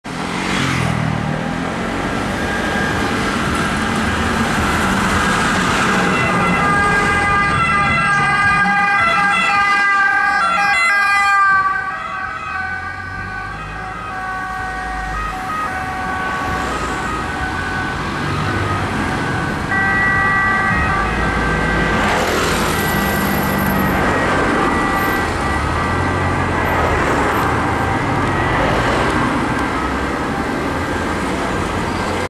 Ambulanza 1-424 Croce Rossa Santo Stefano al Mare + Automedica ALFA 1 in sirena!
L'ambulanza 424 della Croce Rossa Italiana, del comitato di Imperia, UTD di Santo Stefano al Mare si dirige insieme all'automedica ALFA 1 all'ospedale di Imperia in codice rosso di massima gravità dopo un soccorso a San Lorenzo al Mare.
Da notare l'utilizzo dei faretti e della traffic lights integrate nella barra di ALFA 1

Renault Master
Allestimento: Orion

Ford Kuga
Allestimento: Alessi & Becagli